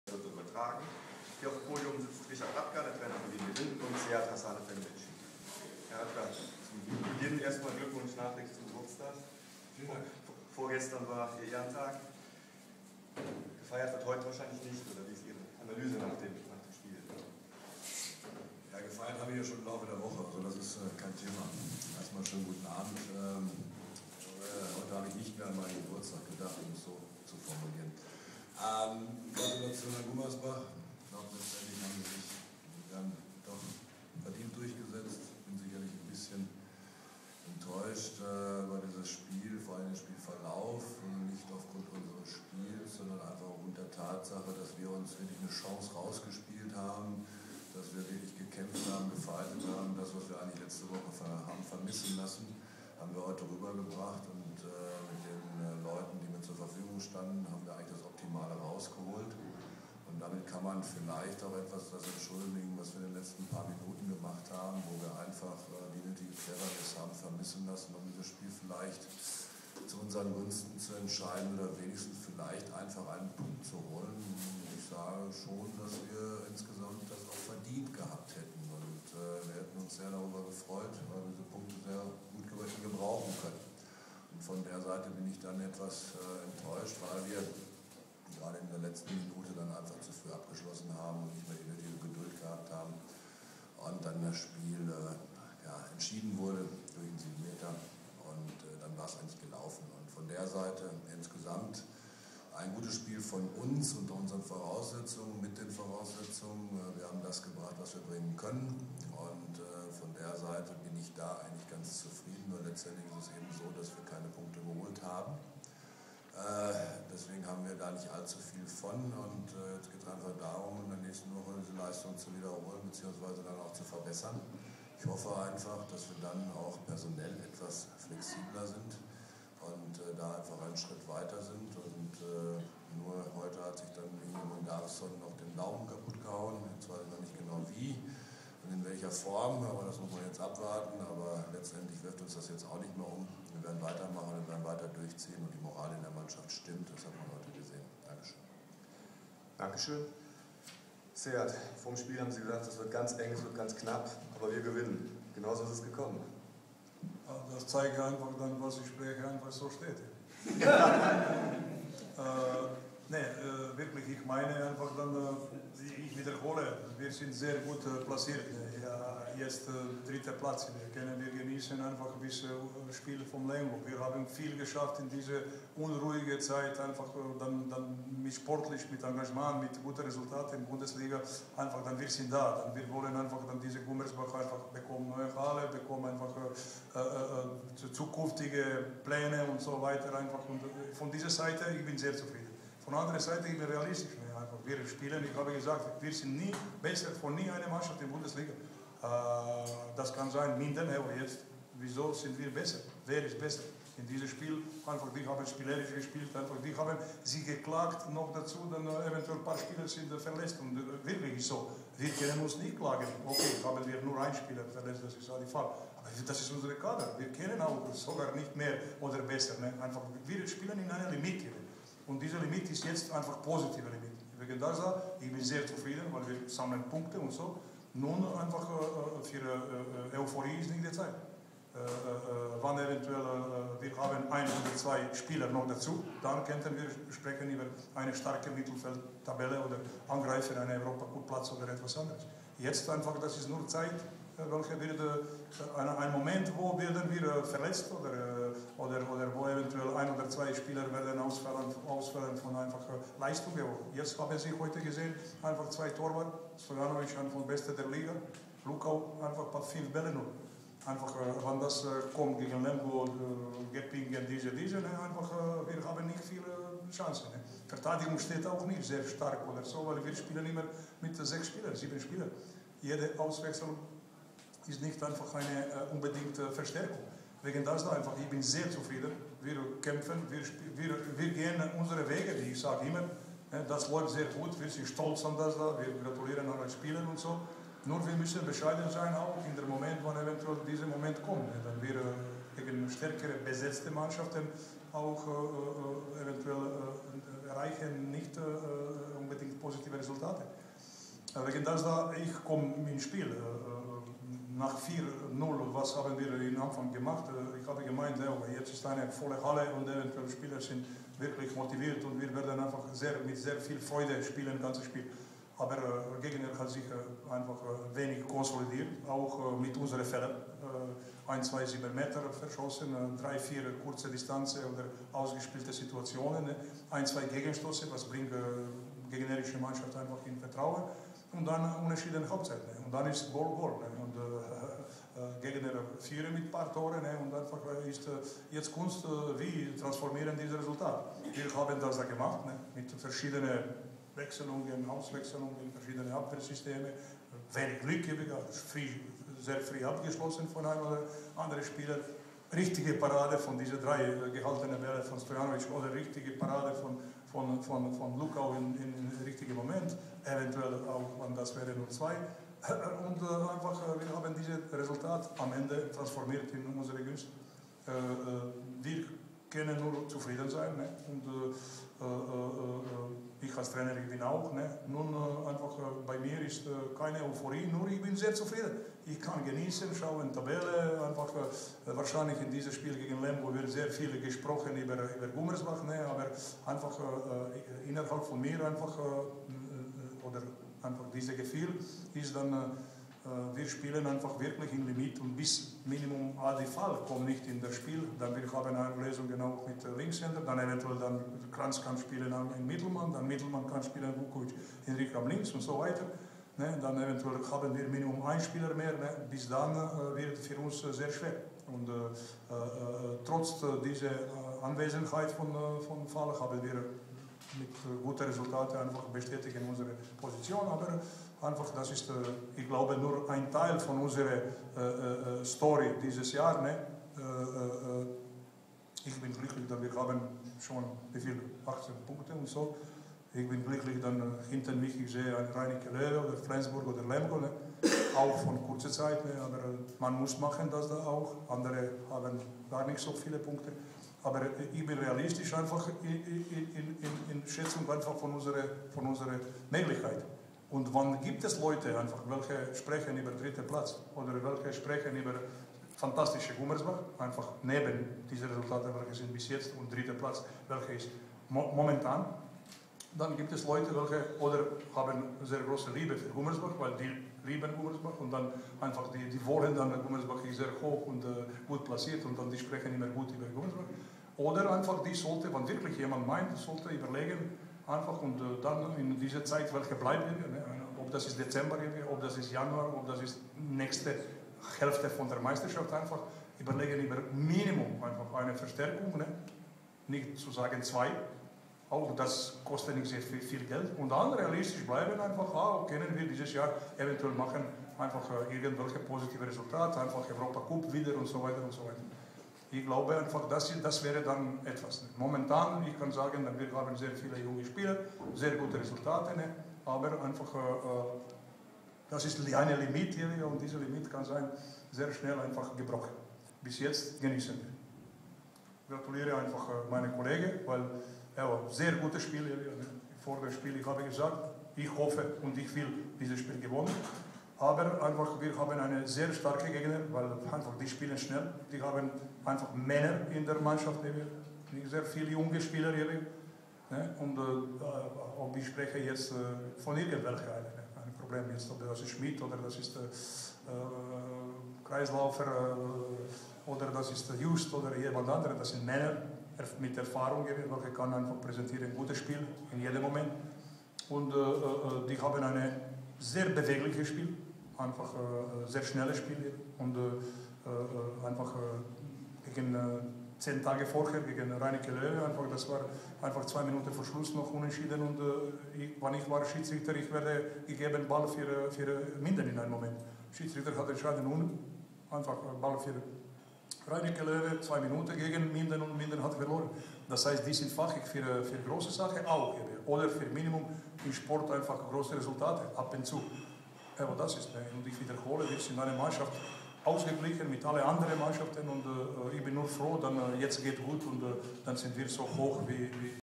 TV.NEWS-Oberberg.de: Pressekonferenz VfL Gummersbach - GWD
TV.NEWS-Oberberg.de:

Pressekonferenz VfL Gummersbachgegen  GWD Minden
Ergebnis 31-29 (HZ 15-15)

Produziert am Sa. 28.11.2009 für

@Eugen Haas Halle, Gummersbach

Nicht für fremde kommerzielle Medien frei gegeben!